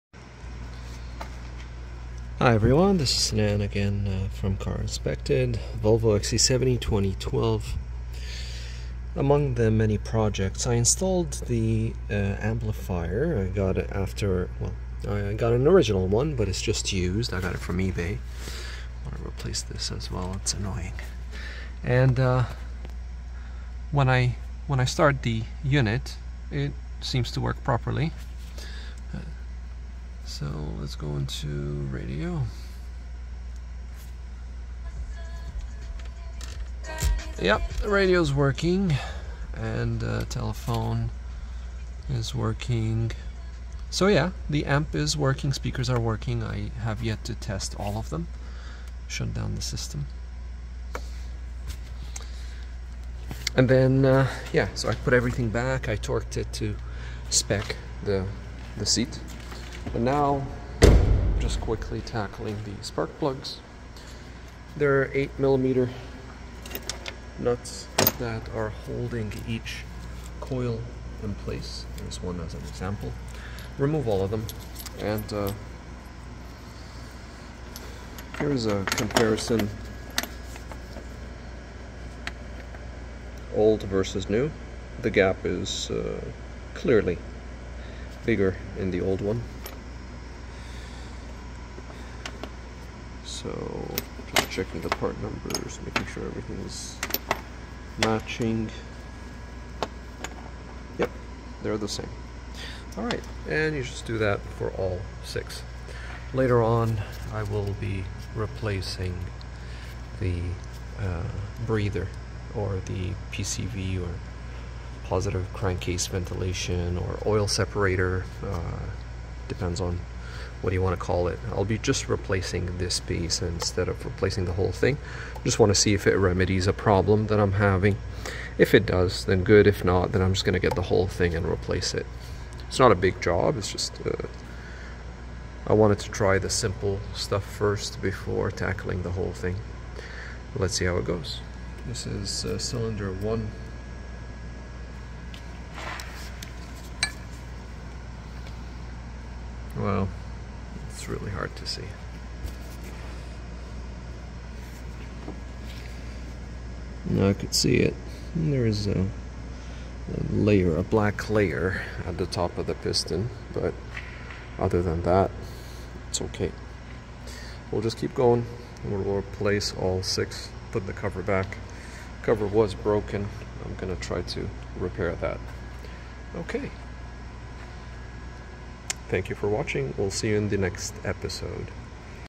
2012 Volvo XC70 DIY modifications. episode 4, Amplifier and spark plugs.
In this video, changed the amplifier and the spark plugs...

Model year 2012 Volvo XC70 premier plus edition,with BLIS (blind spot monitoring), auto wiper, parking sensors for the front and rear, note that the 2012 is the first year that came with the redesigned dash (to accommodate the center screen). no premium sound, no xenon headlights, shifter looks horrid, cluster was damaged (just plastic cover).